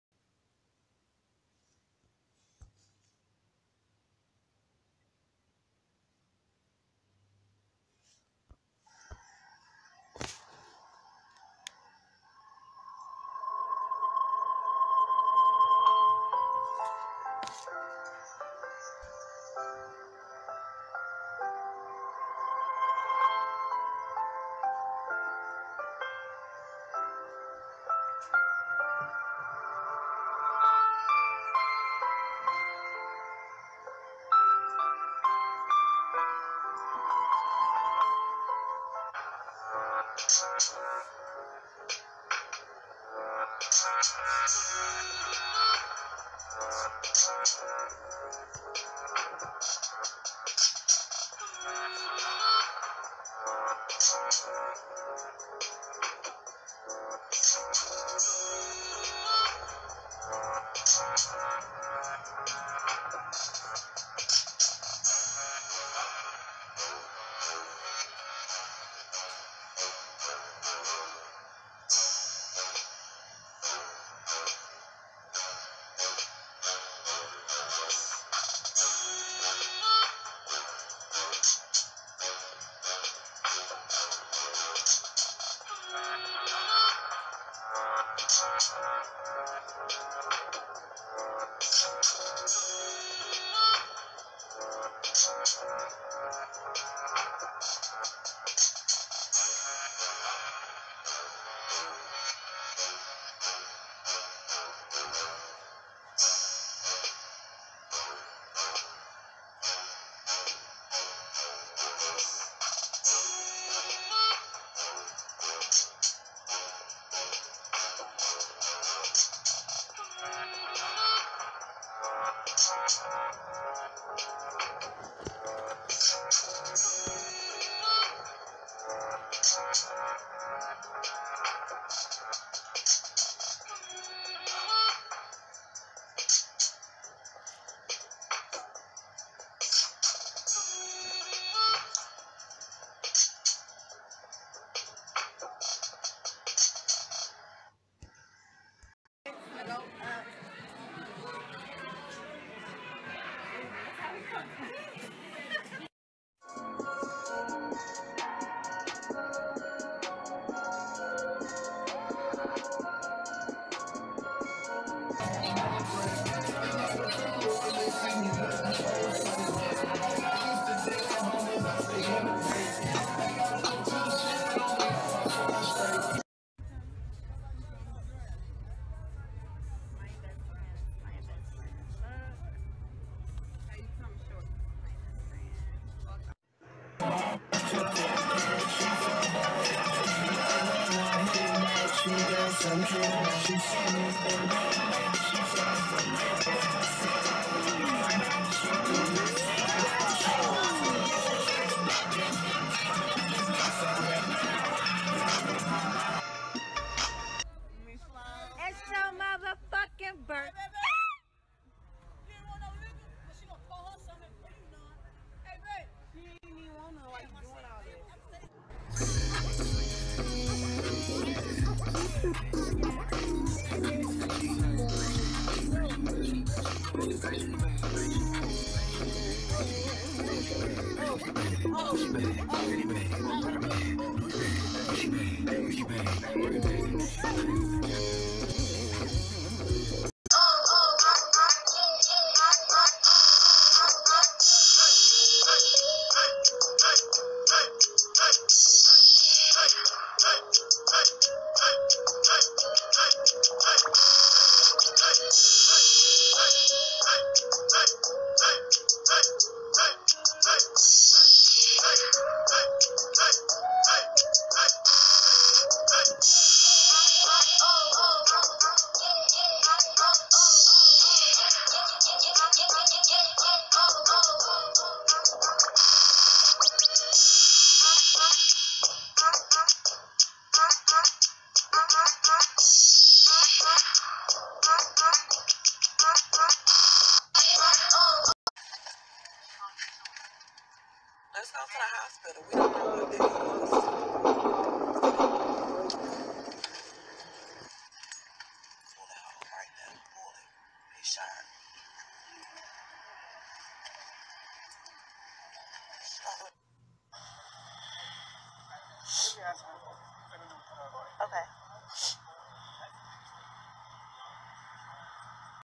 Copy of Another One of the Many Mystery Guest Discovered-From the Crowne Plaza Hotel Party
Your actually getting 2 if you don't know who Kellz Robinson is. But this video is showing the female in the floral ADIDAS.